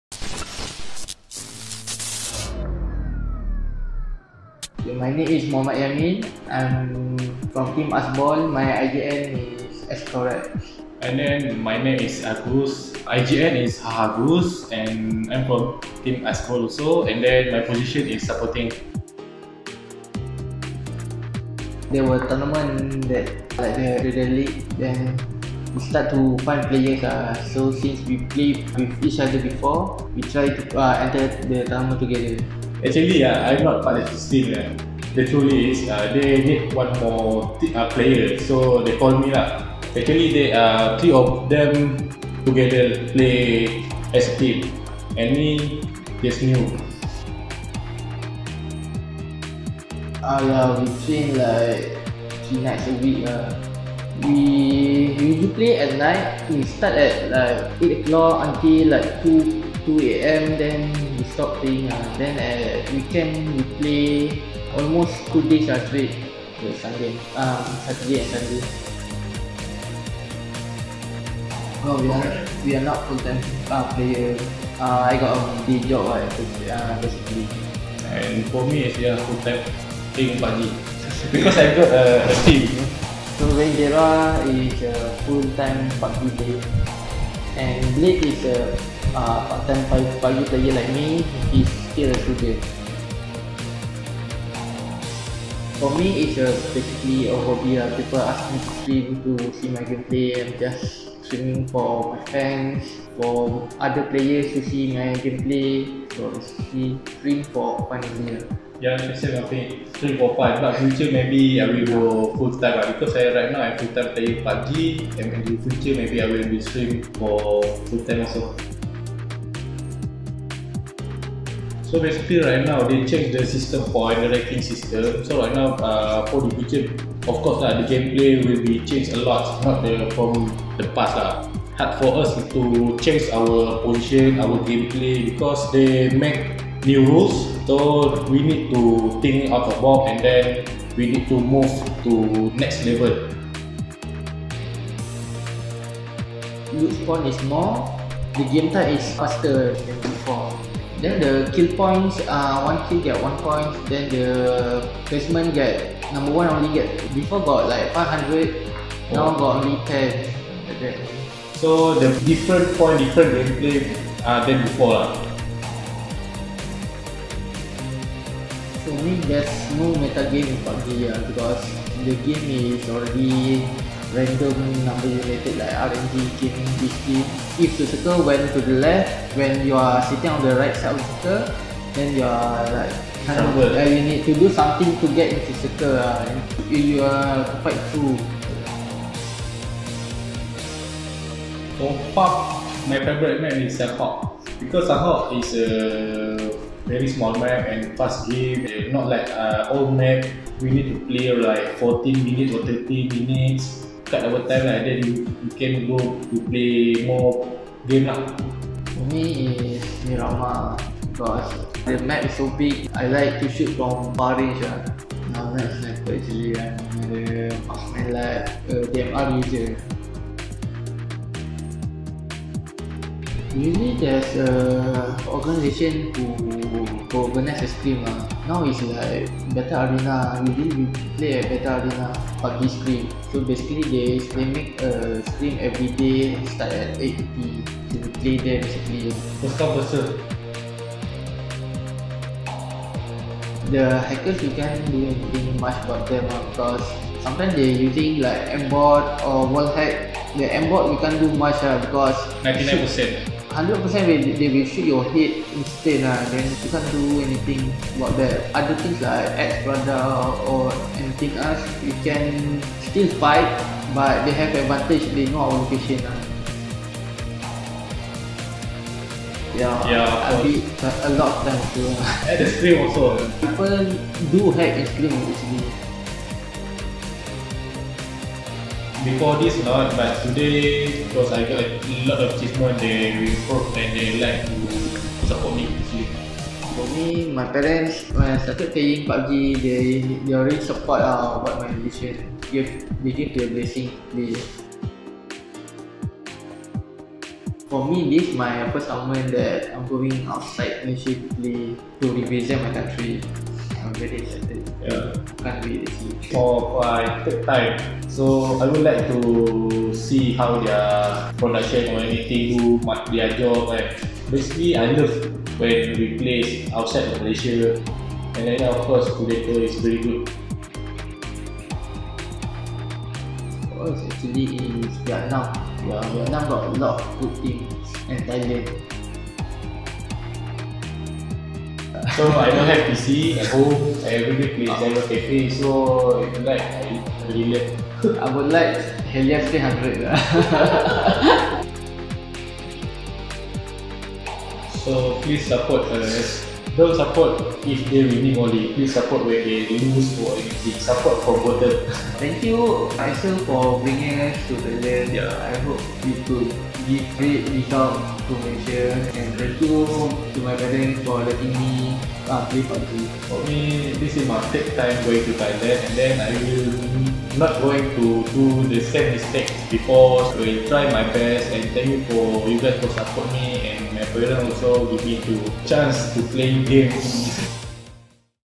Temuramah Bersama Team Asbol (Gamersantai)
Haaa admin kat sini nak tunjuk interview dengan Team Asbol, team PUBG Malaysia yang menang Predator League baru-baru ni tu! Jom kita tengok interview diorang! Video Temuramah ini bawakan oleh Acer Malaysia.

Faceook: Facebook.com/GamerSantai
Twitter Kitorang: Twitter.com/GamerSantaiMY